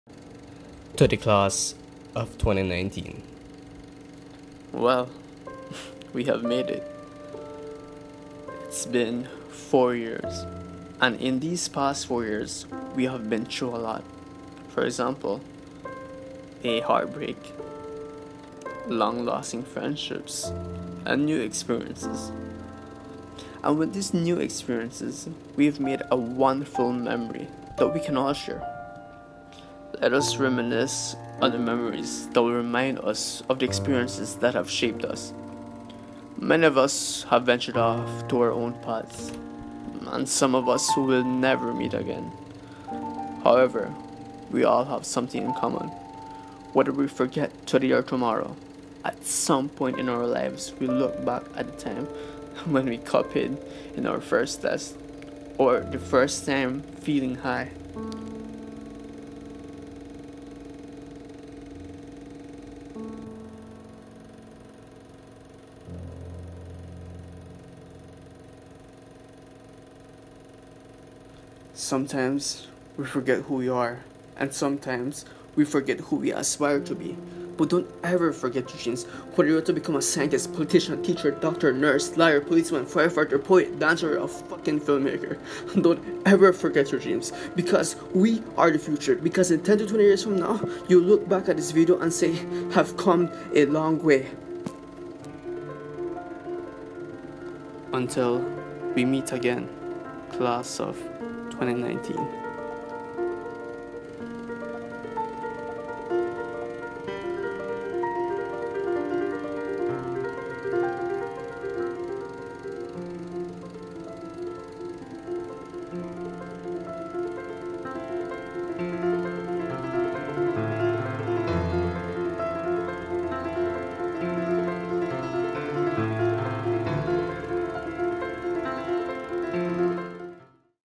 To the class
I was supposed to finish this video a week after graduation but I never did. Last night, I was just chilling and I heard the song again and I felt inspired to finish it. The voice over is not great lol cuz everything came out straight out of my head in one recording. I am aware of grammatical mistakes.